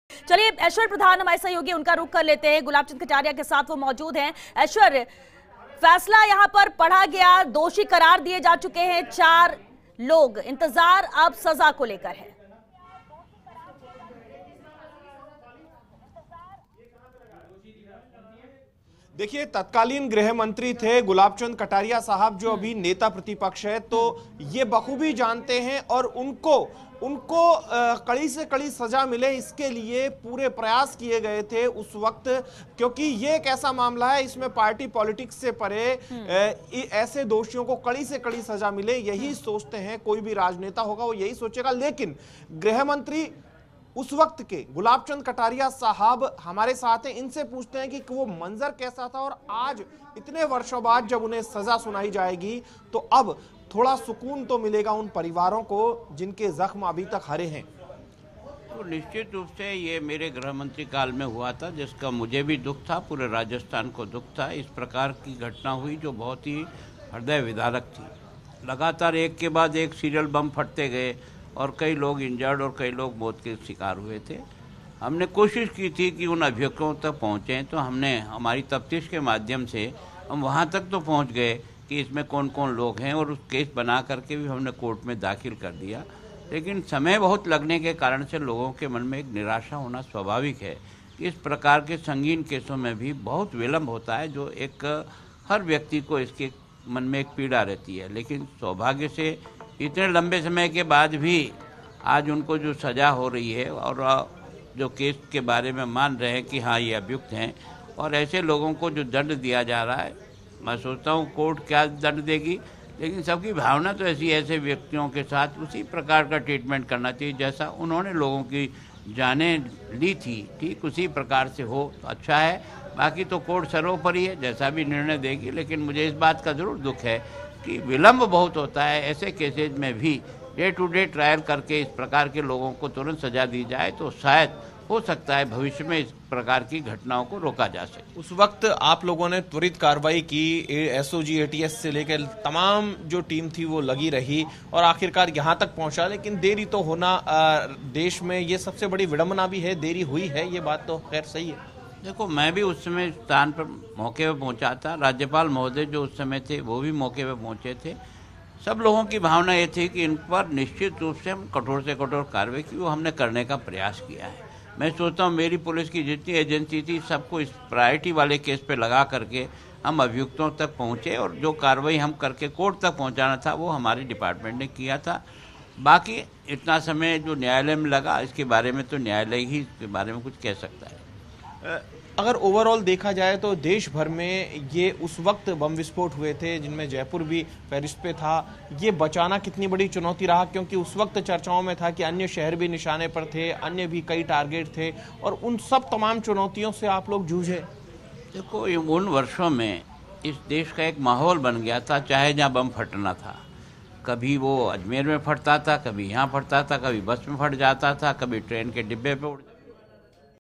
Jaipur Bomb Blast Case फैसले पर Gulab Chand Kataria की प्रतिक्रिया
About this Video:
Gulab Chand Kataria's reaction to the Jaipur Bomb Blast Case verdict